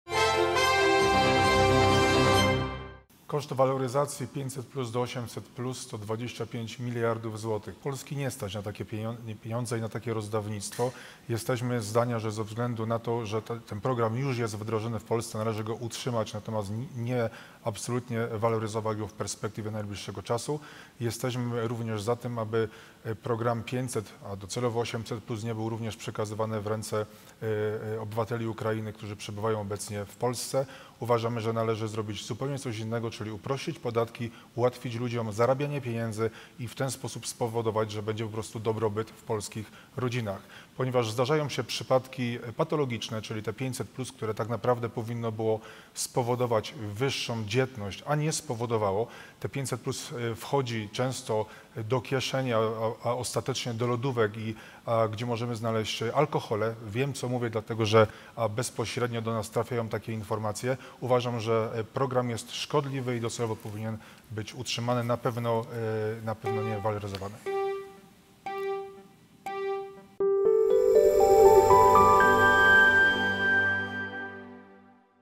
Grzegorz Płaczek, Konfederacja: 500+? Pieniądze trafiają do lodówek
Grzegorz Płaczek, Konfederacja, w debacie TVS mówi o tym, czy ta partia utrzyma program 500+. - To szkodliwy program. Koszty waloryzacji programu 500+są do 800+ horrendalnie wysokie. Uważamy, że należy wprowadzić coś innego. Lepiej ułatwić ludziom zarabianie pieniędzy, obniżyć podatki.

Najważniejsi kandydaci do sejmu z okręgu katowickiego (okręg 31) w studiu TVS w piątek 6.10 o godz. 20 mówią, jakie mają pomysły na gospodarkę, mieszkalnictwo, uchodźców i politykę senioralną.
W studiu: Monika Rosa (Koalicja Obywatelska), Michał Gramatyka (Trzecia Droga), Maciej Konieczny (Lewica), Grzegorz Płaczek (Konfederacja) i Marek Bartoń (Bezpartyjni Samorządowcy)